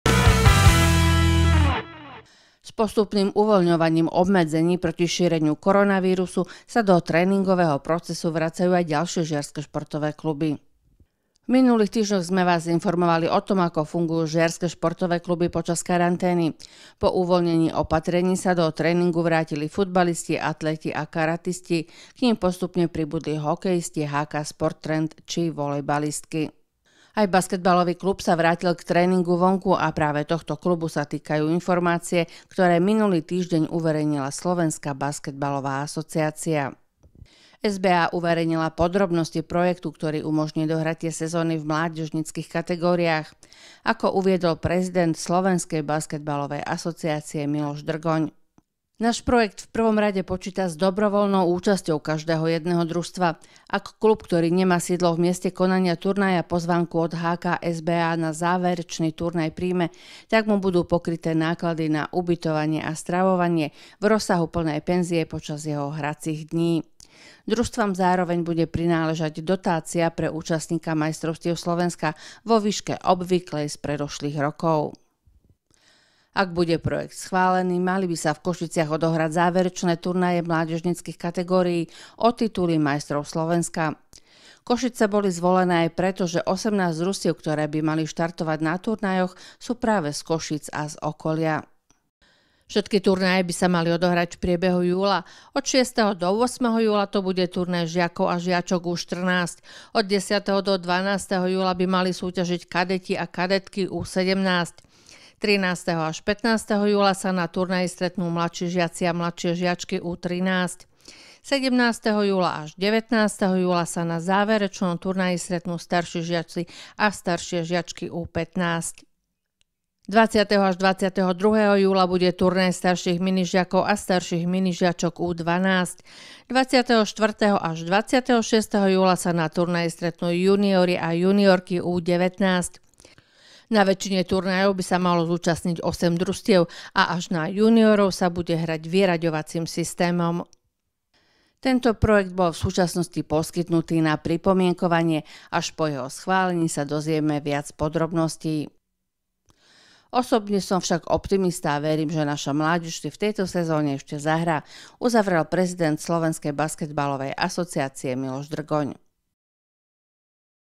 Žiarskych basketbalistov čakajú už v júli turnaje
Vieme termíny turnajov jednotlivých kategórii.